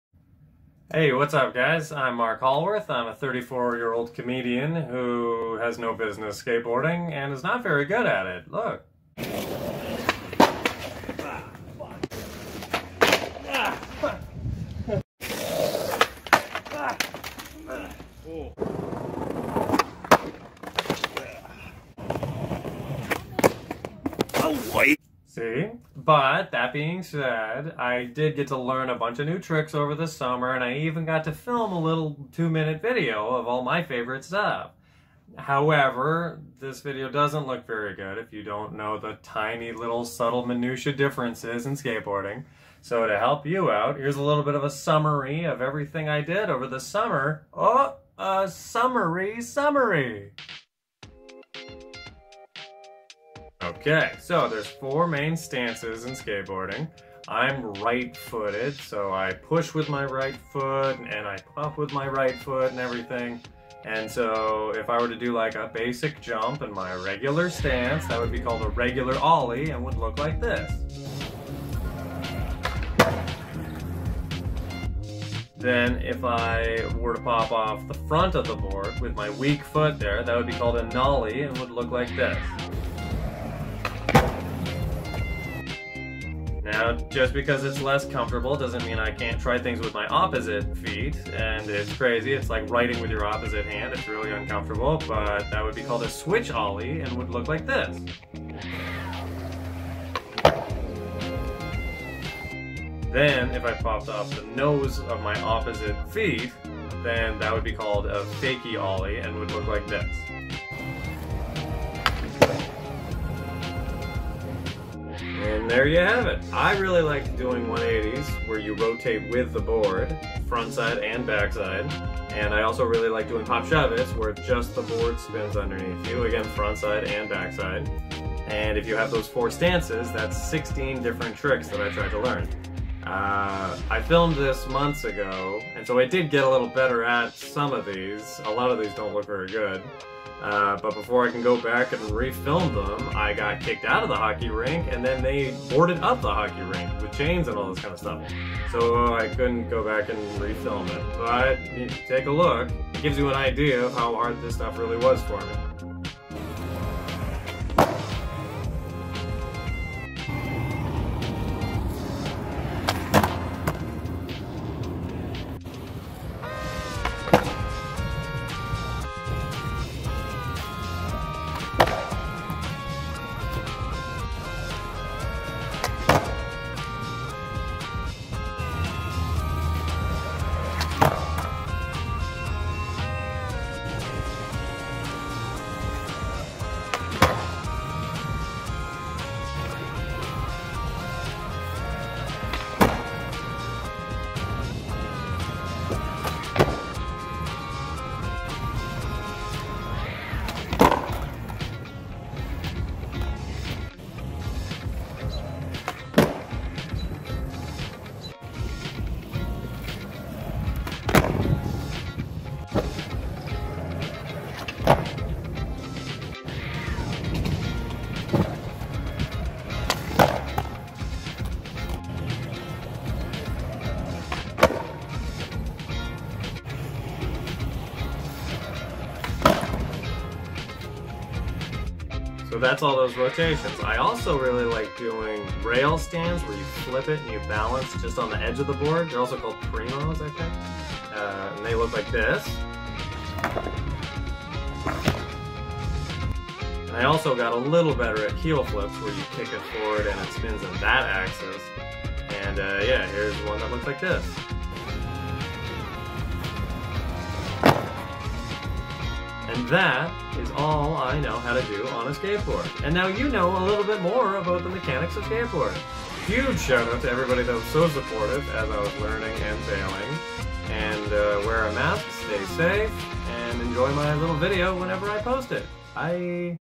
Summer y Summary
A description of all the tricks Marc Hallworth knows how to do!